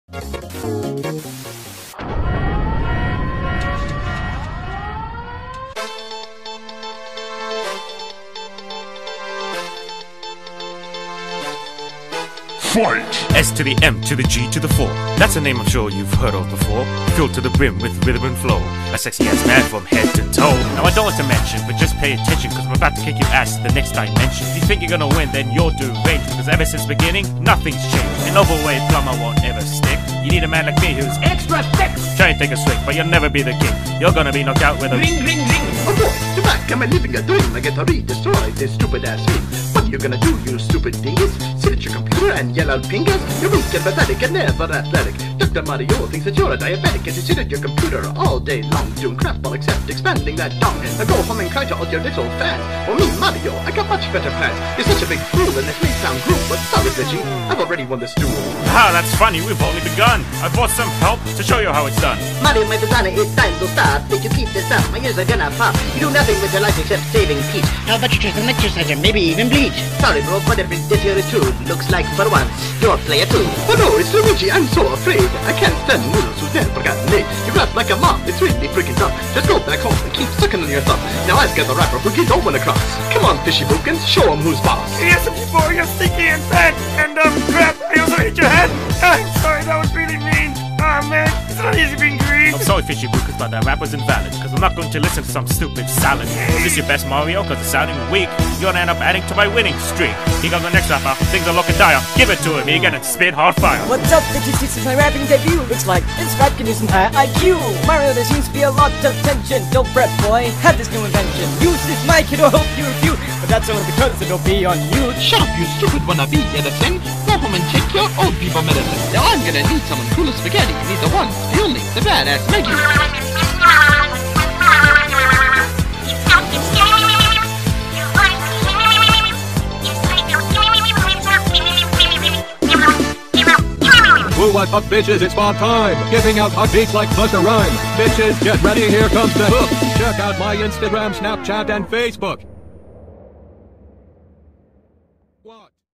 Mario vs smg4 round 3 with roast bars
Spoiler alert SMG4 won